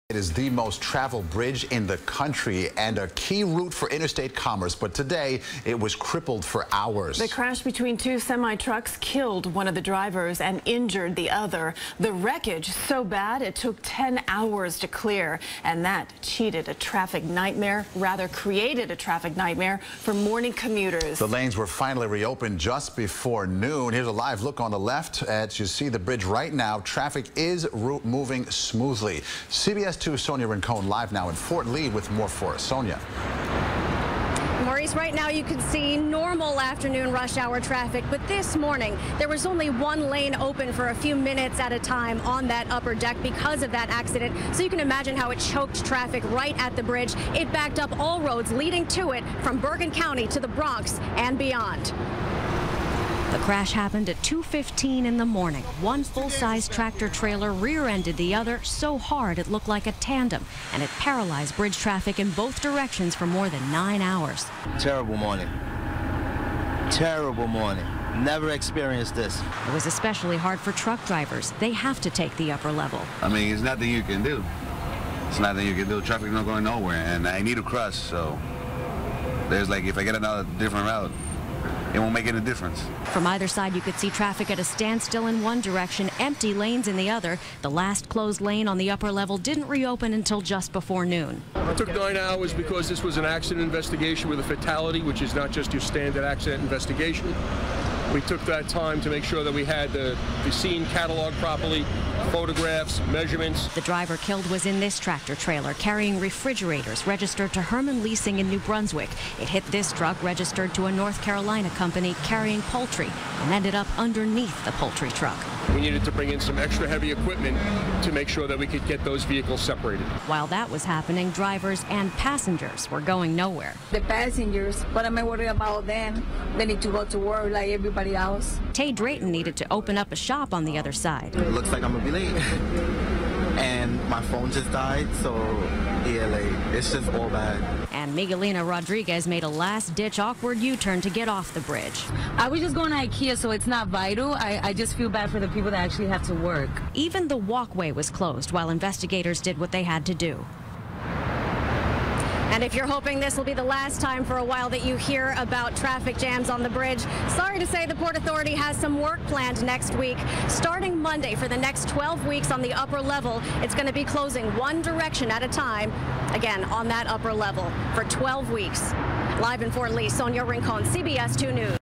Deadly Crash Snarls Traffic For Hours On George Washington Bridge
All lanes have reopened on the upper level of the George Washington Bridge after a fatal crash snarled traffic for hours Thursday morning. The crash happened shortly after 2 a.m., authorities said. The driver in the rear truck died when the vehicle became wedged under the other truck's trailer, Port Authority Police Capt. Ron Shindel said. CBS 2's Sonia Rincon reports.